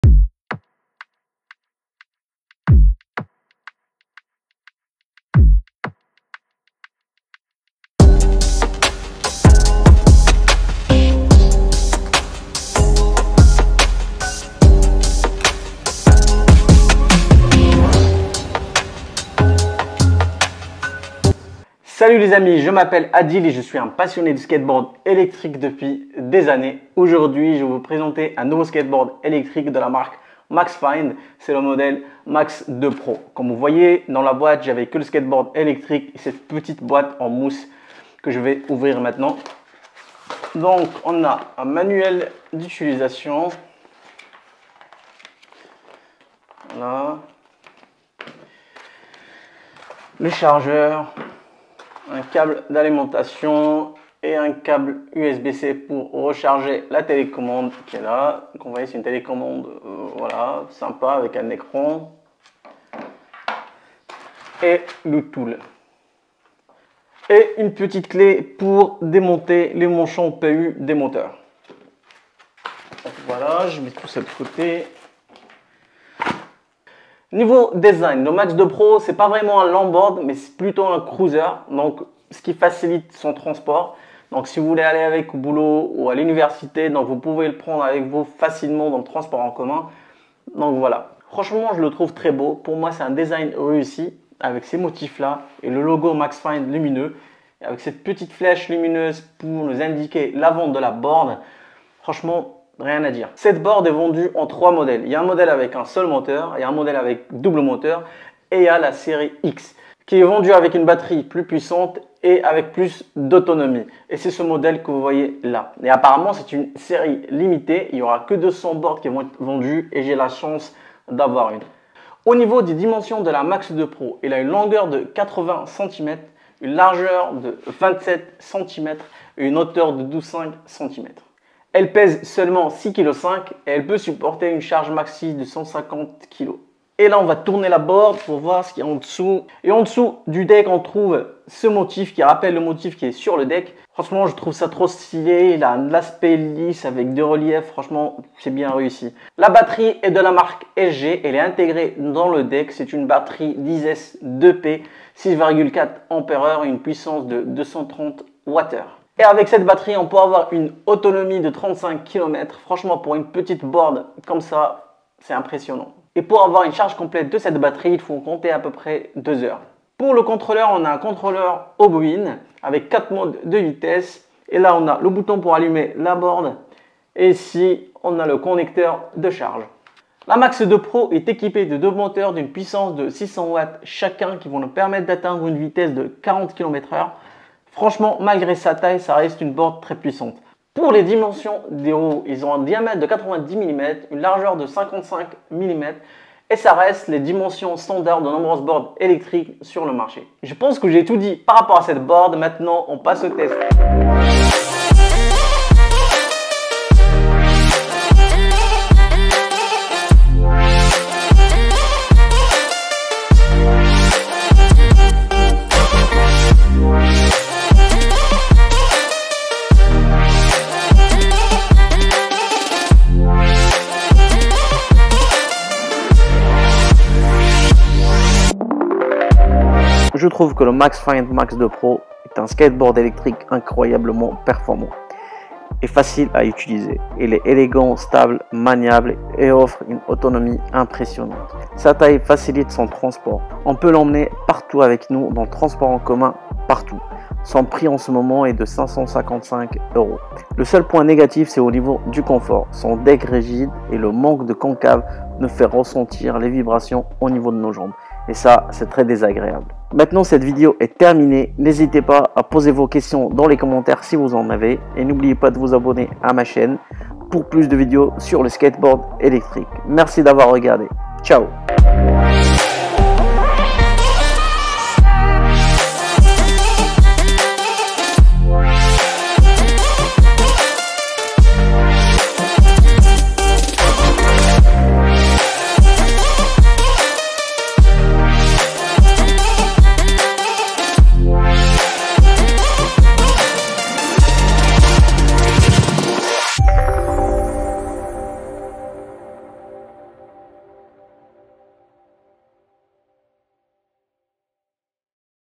MAXFIND MAX2 PRO le petit skateboard électrique avec une grande autonomie
Dans cette vidéo, je vous présente en détail le skateboard électrique MAXFIND MAX2 PRO, une véritable révolution dans le monde de la mobilité électrique. Ce skateboard combine à la perfection performance, style et polyvalence pour offrir une expérience de conduite exceptionnelle.

Le MAXFIND MAX2 PRO est équipé d'un moteur puissant qui offre une accélération impressionnante, vous permettant de vous déplacer rapidement et en douceur. Sa batterie longue durée vous garantit des trajets prolongés sans avoir à vous soucier de la recharge fréquente. Les roues robustes assurent une adhérence optimale sur différents types de surfaces, que vous soyez en ville ou sur des routes plus accidentées.

La télécommande ergonomique vous donne un contrôle total sur votre vitesse et votre direction, offrant une expérience de conduite intuitive. De plus, le MAX2 PRO est conçu pour être portable et léger, ce qui en fait le compagnon idéal pour les déplacements urbains.

Dans cette vidéo, je vous montre non seulement les caractéristiques techniques du MAXFIND MAX2 PRO, mais je partage également mon expérience personnelle de conduite. Vous découvrirez pourquoi ce skateboard électrique est devenu mon choix préféré pour mes déplacements quotidiens.

N'oubliez pas de regarder la vidéo complète pour obtenir tous les détails et les avantages de ce skateboard électrique révolutionnaire. Si vous recherchez une solution de mobilité électrique pratique et performante, le MAXFIND MAX2 PRO pourrait bien être la réponse à vos besoins.